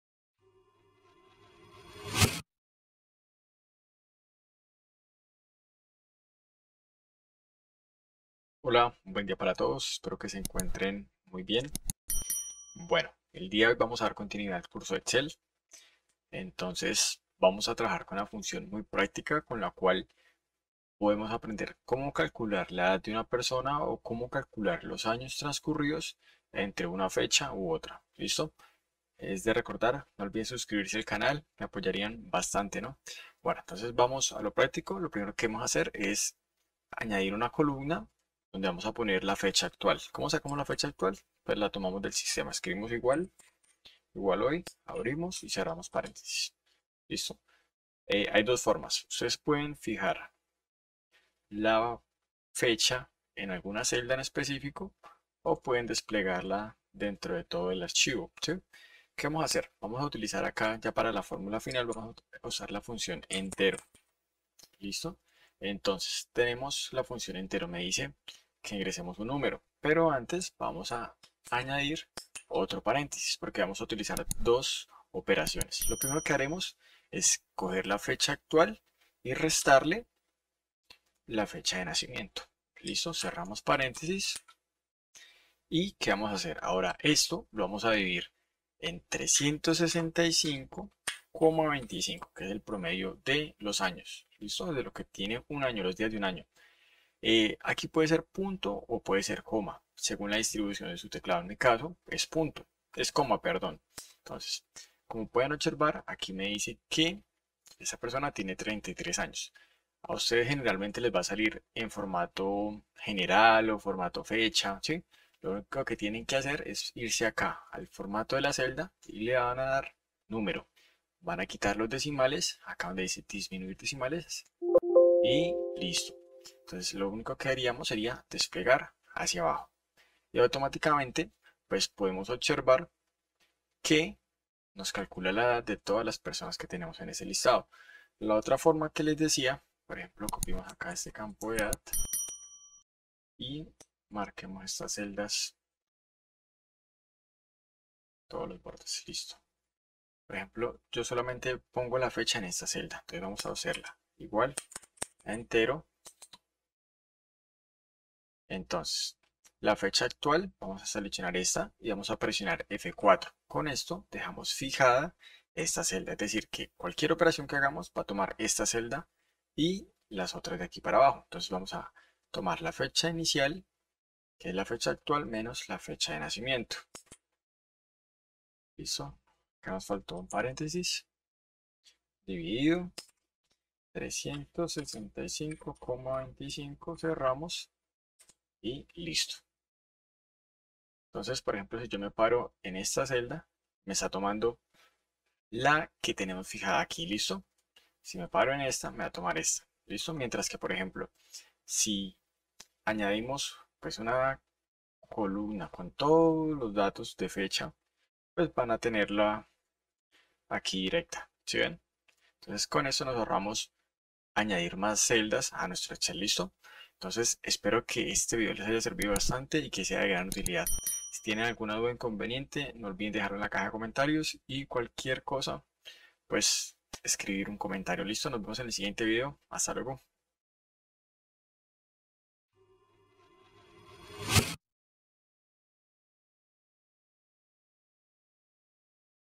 Como calcular la edad en años en Excel
Hola, el día de hoy les explicaré como calcular los años transcurridos entre dos  fechas en Excel, usando la función entero.